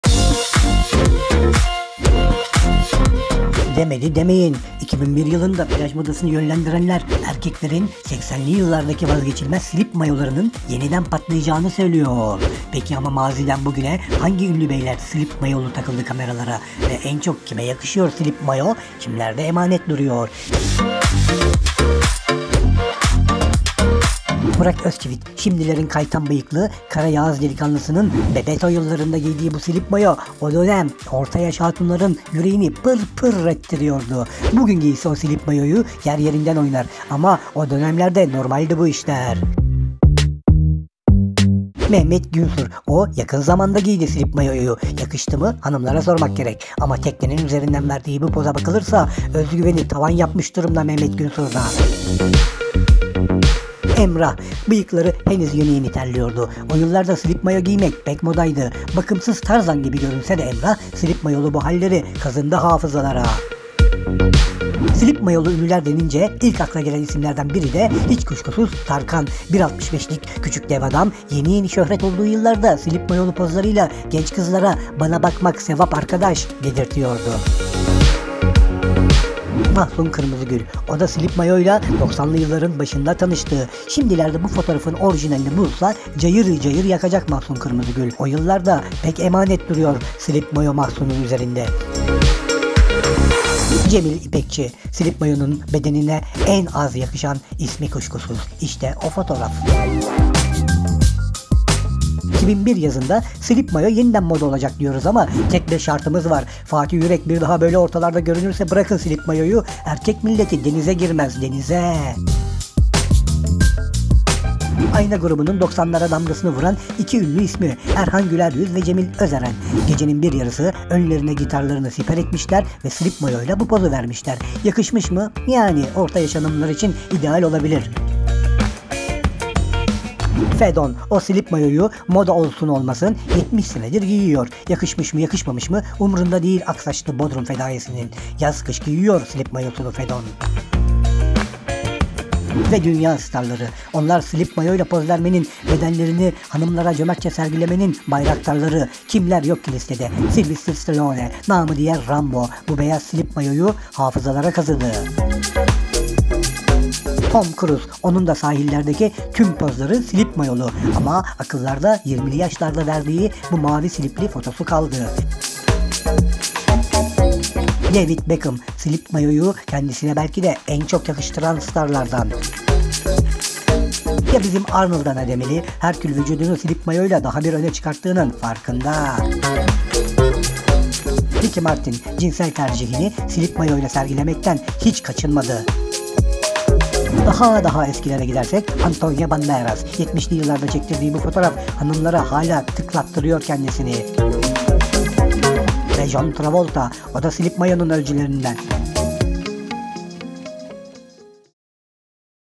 Slip Mayolu Ünlüler | MAZİDEKİ SLİP MAYOLAR 2021 YILINDA GERİ Mİ DÖNÜYOR?
2021 yılında plaj modasını yönlendirenler, erkeklerin 80’lerdeki vazgeçilmezi slip mayoların patlayacağını söylüyor. Peki ama maziden bugüne hangi ünlü beyler slip mayolu takıldı kameralara? Ve en çok kime yakışıyor slip mayo, kimde emanet duruyor. İşte 2021 yazında yenisini göremeceğimiz Burak Özçivit’ten Mehmet Günsür’e, Emrah’tan Tarkan’a, Mahsun Kırmızıgül’den Cemil İpekçi’ye, Fatih Ürek’ten Kadir İnanır’a, Erhan Güleryüz ve Cemil Özeren’den Fedon’a kadar bir çok ismin eski slip mayolu halleri…